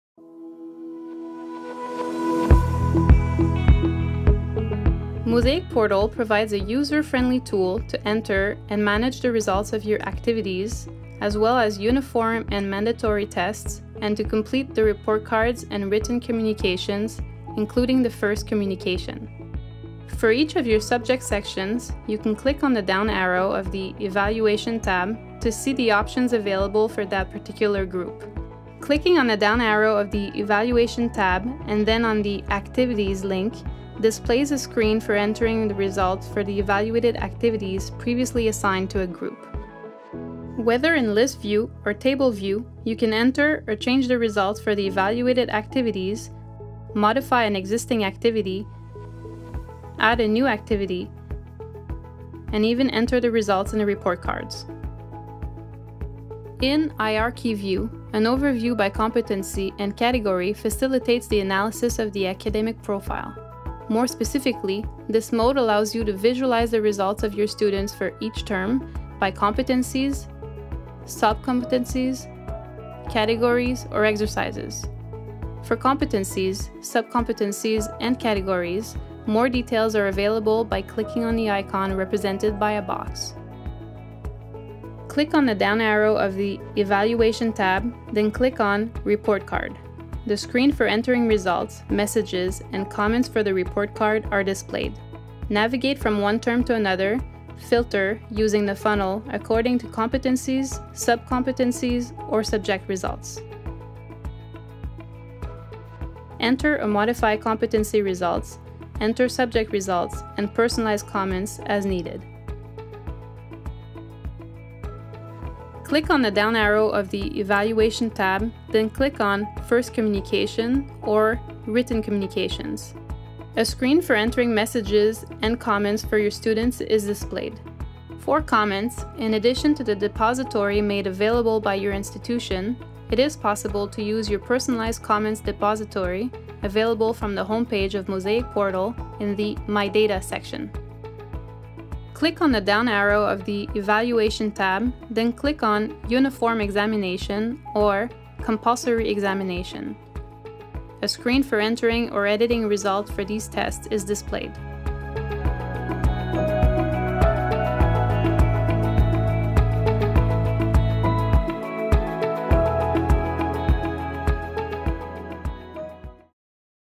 Introduction to the evaluation tools - Mozaïk-Portal For Teachers
**Warning** : The functionalities presented in this capsule may differ from those of your school due to software updates or the lack of activation of certain functionalities by your school organization.

Mozaïk-Portal provides a user friendly tool to enter and manage the results your activities, uniform and mandatory tests, to complete the report card and written communications.